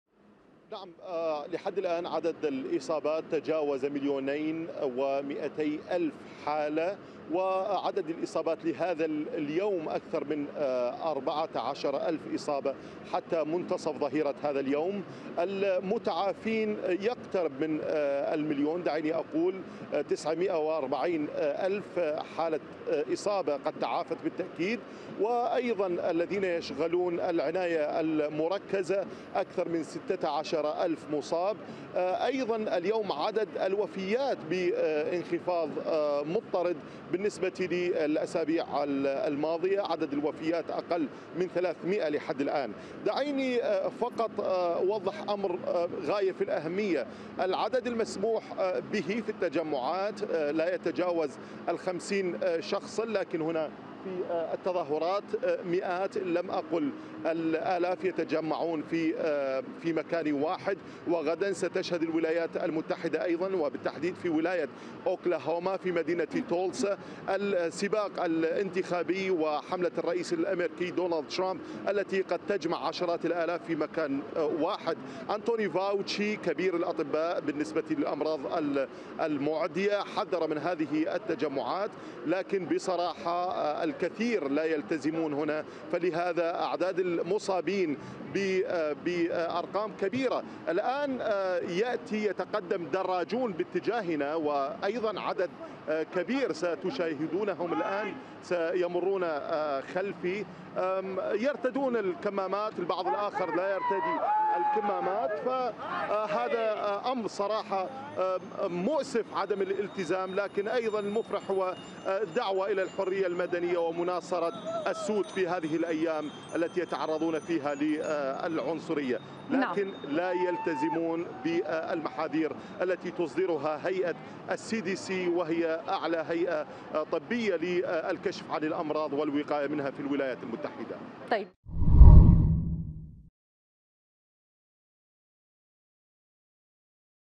عدم الالتزام في الولايات المتحدة يرفع من أعداد المصابين بفيروس كورونا
ارتفعت عدد الإصابات في الولايات المتحدة بفيروس كورونا.
مزيد من التفاصيل مع مراسل الغد في واشنطن عمر محمد.